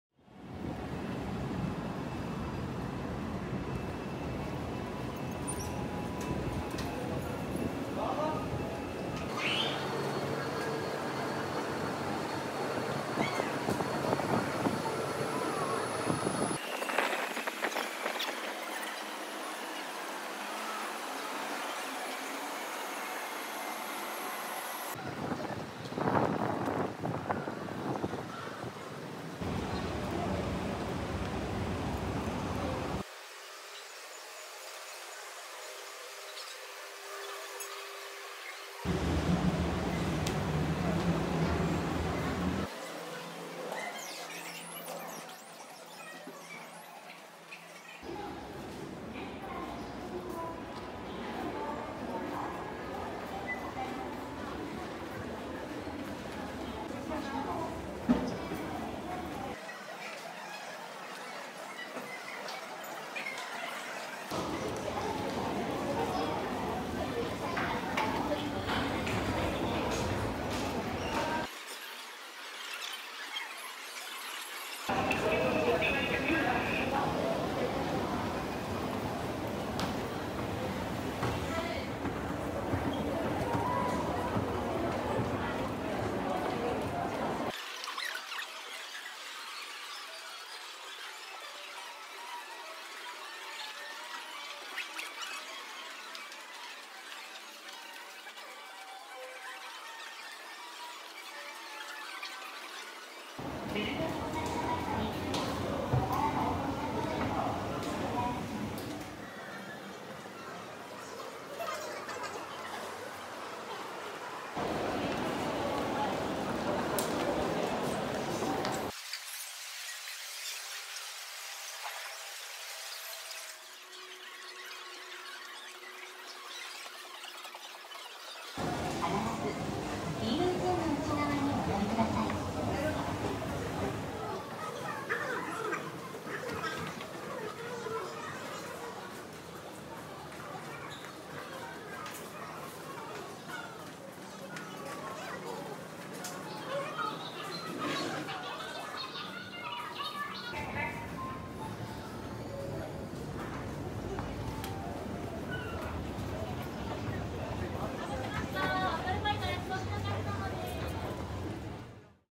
大宮駅構内の混雑を避けて西口から東口への行き方〜ルミネ3F経由
大宮駅構内は混んでいるので、ルミネ3Fを通って西口から東口に行ってみました。

2021年8月撮影